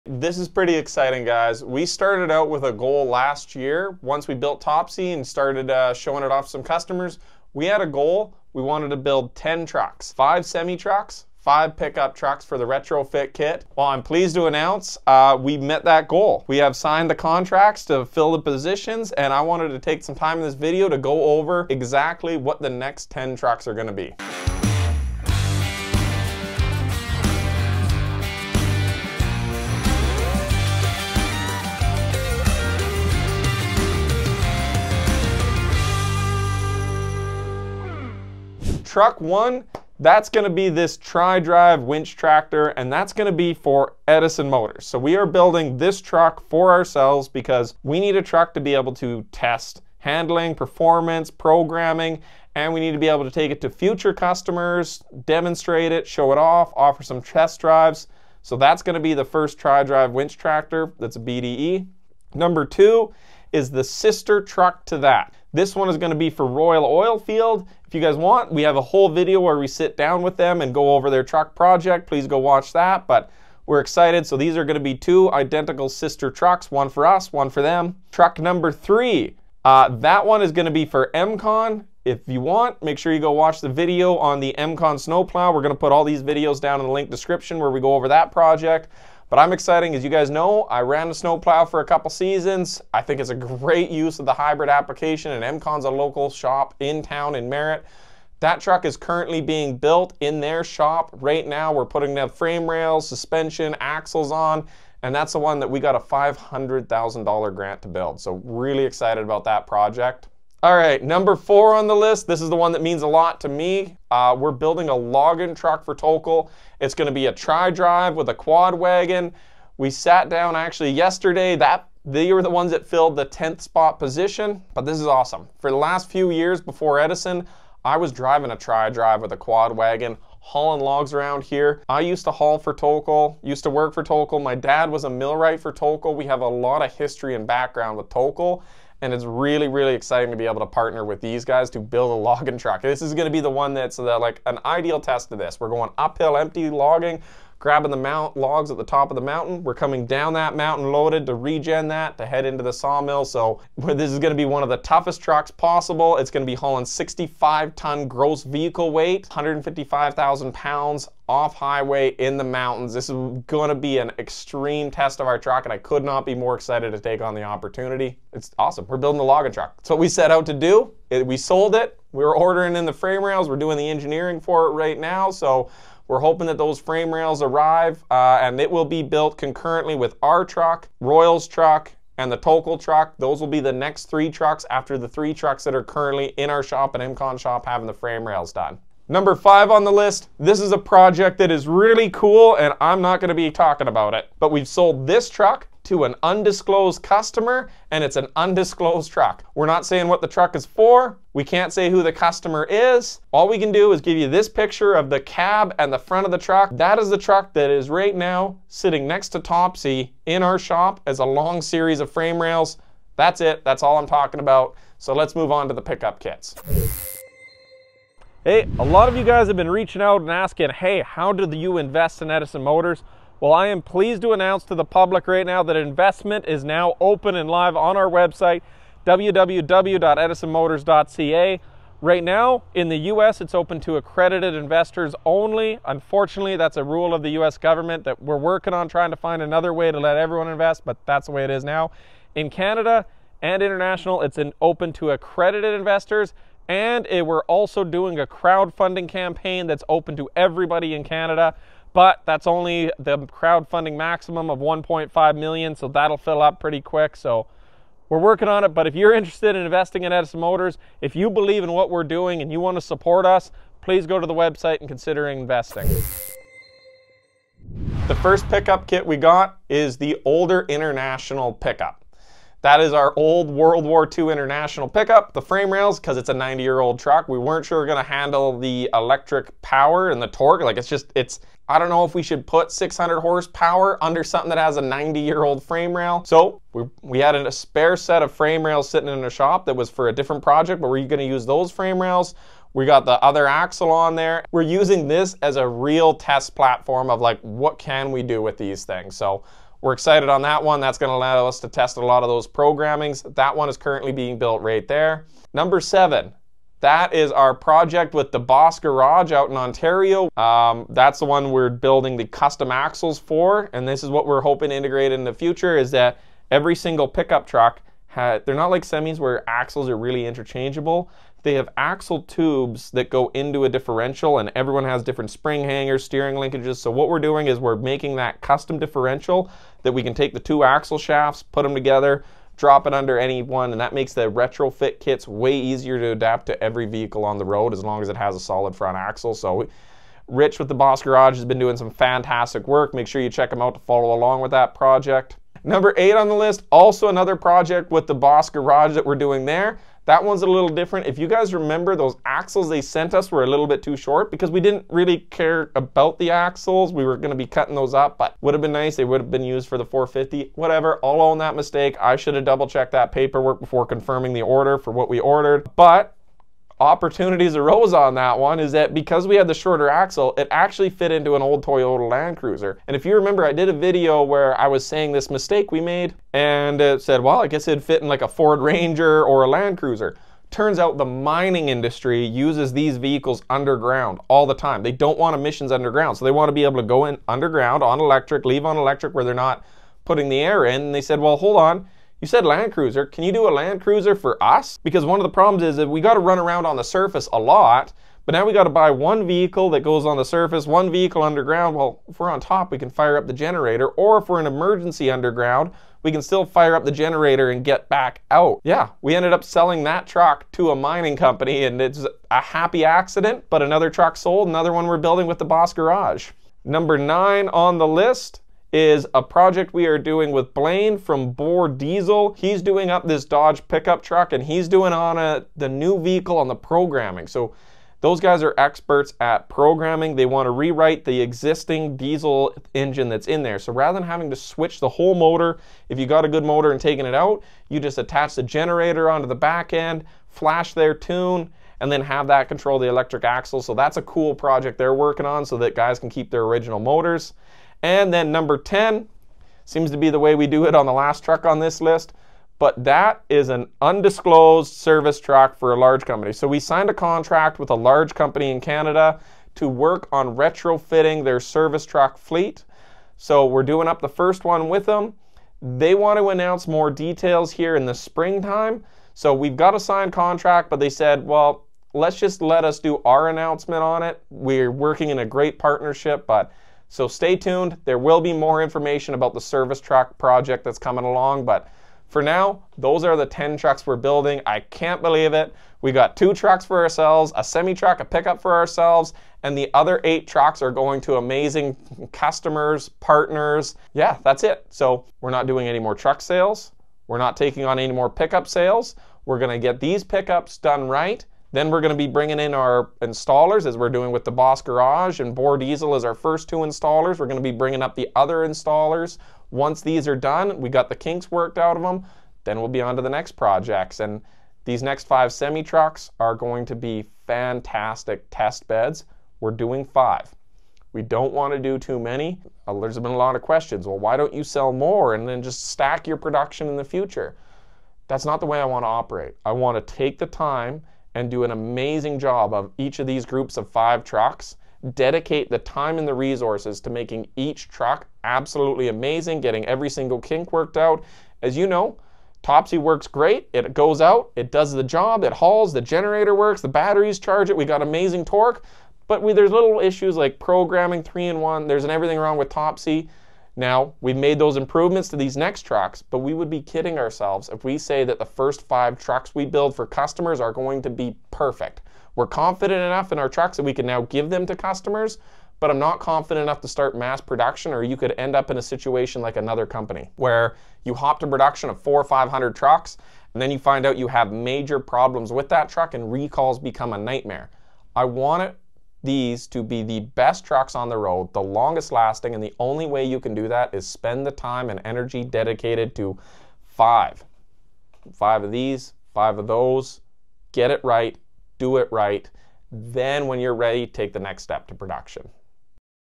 Edison Motors’ 10 New Hybrid Truck Builds & Future Plans!
We started with a goal at Edison Motors: to build 10 Diesel Electric Trucks – 5 semi-trucks and 5 pickup trucks with retrofit kits. We’re thrilled to announce that we’ve met that goal!

In this video, we break down the projects for our next 10 trucks, including:
 • Tri-Drive Winch Tractor for in-house testing
 • Custom builds for Royal Oilfield, Emcon, Tolko, and a secret truck
 • Exciting collaborations with The Boss Garage and Bore Diesel
 • Undisclosed service truck projects for a major company

We also discuss the progress of our hybrid retrofit kits for pickups and semis, share details about our partnerships, and open up investment opportunities for those passionate about transforming the future of transport!

00:00 - 00:38 Intro
00:38 - 01:01 Truck #1, Edison Winch Tractor
01:01 - 01:17 Truck #2, Royal OilField Winch Tractor
01:17 - 01:50 Truck #3, Emcon Snow Plow
01:50 - 03:08 Truck #4, Tolko Logging Truck
03:08 - 03:36 Truck #5, It’s a Secret
03:36 - 04:26 Investment is now open
04:26 - 05:05 Truck #6, International K8
05:05 - 05:45 Truck #7, 1995 Dodge Ram
05:45 - 06:56 Truck #8, 2008 Land Cruiser
06:56 - 07:27 Truck #9, Dodge Ram 2500
07:27 - 08:04 Truck #10, Service Truck
08:04 - 09:55 Wrap Up